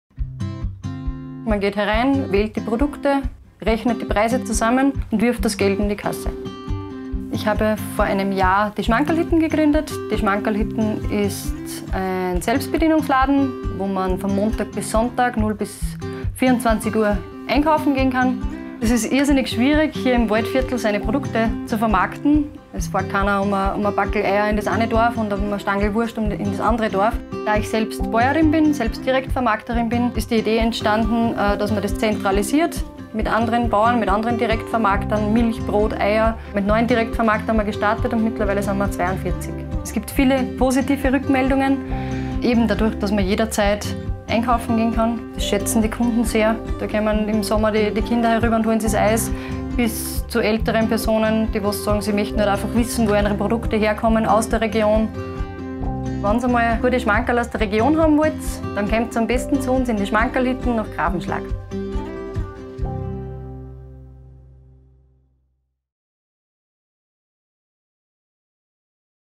Schmankerl Hittn
In der Schmankerl Hittn ist das Einkaufen regionaler Produkte ganz einfach. Man wählt die Produkte aus, rechnet die Preise zusammen und wirft das Geld in die Kasse ein.